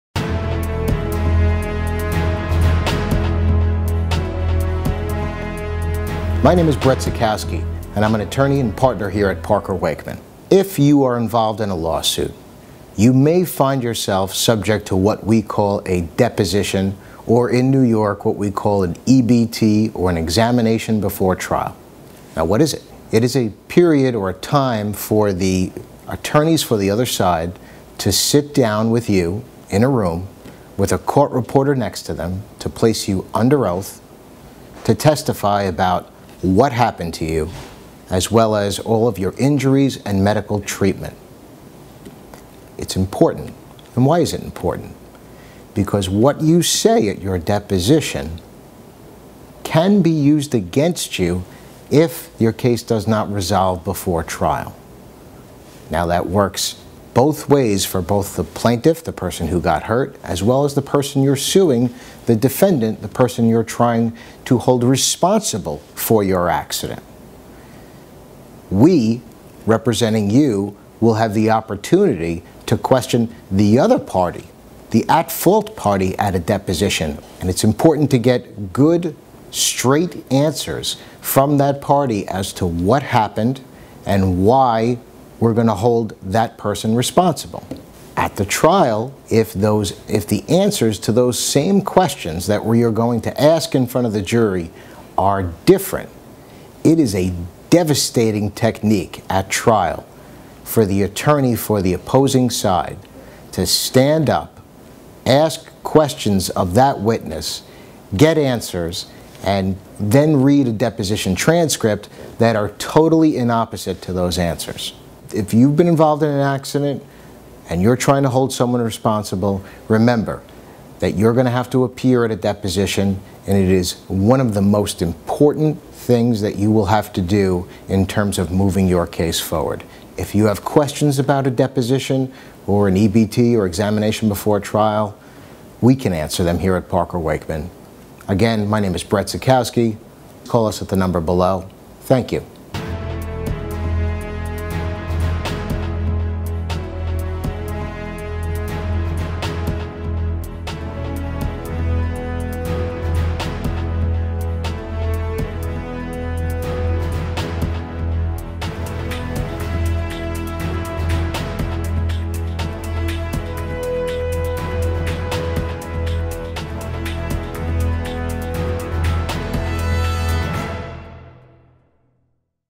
What Is Examination Before Trial – EBT – Or Deposition? – NY Injury Attorney Brett Zekowski
What Is Examination Before Trial – EBT – Or Deposition? – NY Injury Attorney Brett Zekowski explains more

Watch as Brett Zekowski explains Examination Before Trial (EBT), also known as a deposition.

When you are involved in a lawsuit, you may need to take part in what is known as a deposition.  In New York, this is also called Examination Before Trial, or EBT.  This is a time when you and your attorneys, along with the attorneys for the other side sit down alongside a court reporter, and you will be asked several questions under oath.

Your answers to these questions are very important, and your answers can be used against you in court if your case is not resolved before trial.  Both the plaintiff and the defendant are subject to the use of their deposition answers against them in court.  If we are representing you as the plaintiff, we will also have the opportunity to question the defendant you are suing in the same manner.

If answers given during a trial do not match deposition answers provided previously, it can be devastating to a case.  That is why it is very important that your EBT answers under oath be truthful.  Watch the video to learn more.